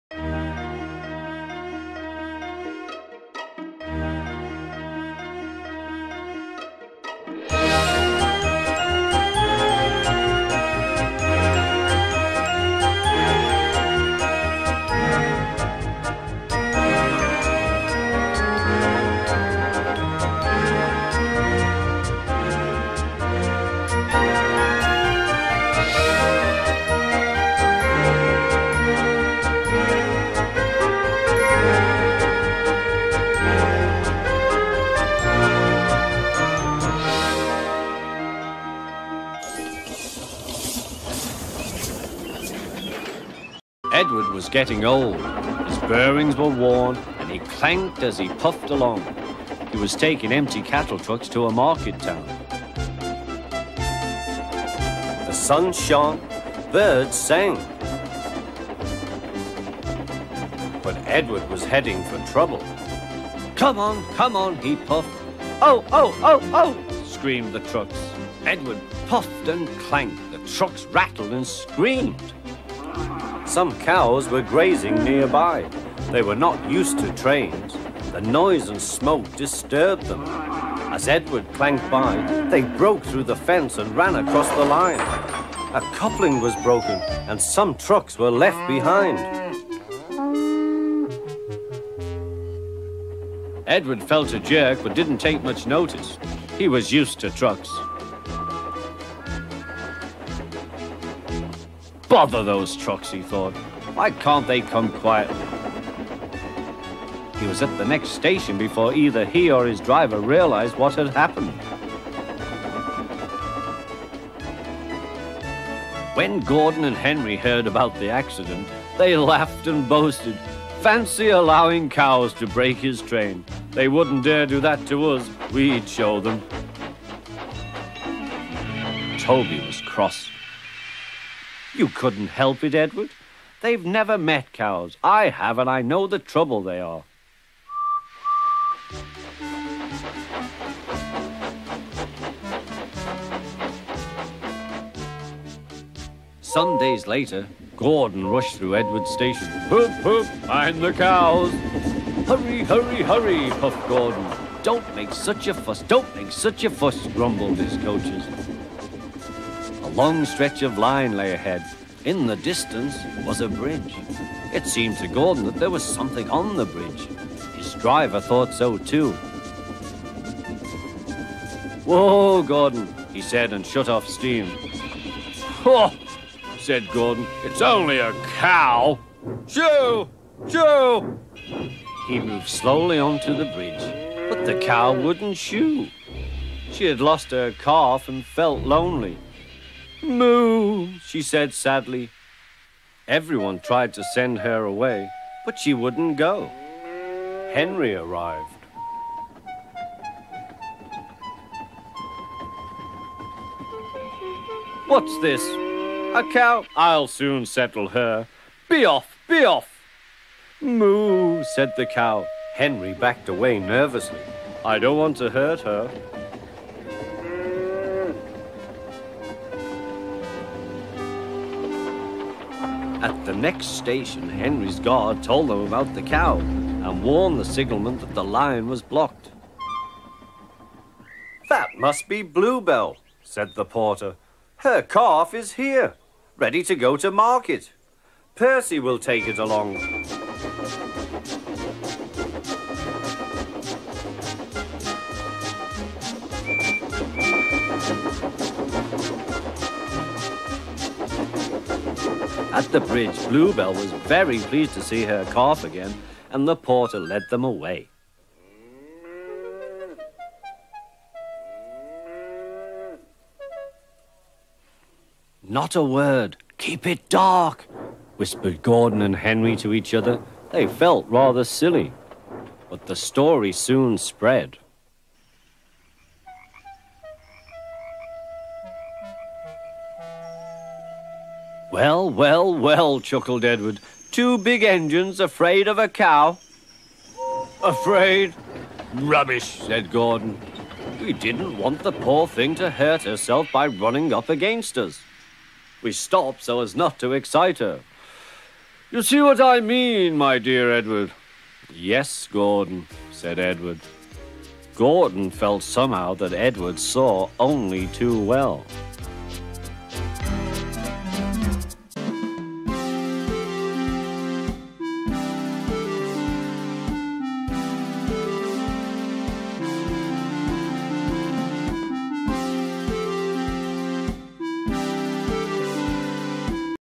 Cows
Edward the Blue Engine & Gordon the Big Engine get into a few incidents involving cows disturbing their work.

Edward the Blue Engine
A Cow On The Line
HO/OO

This video is for GENERAL AUDIENCE. Both  kids & adults can enjoy this video from a nostalgic and personal enjoyment.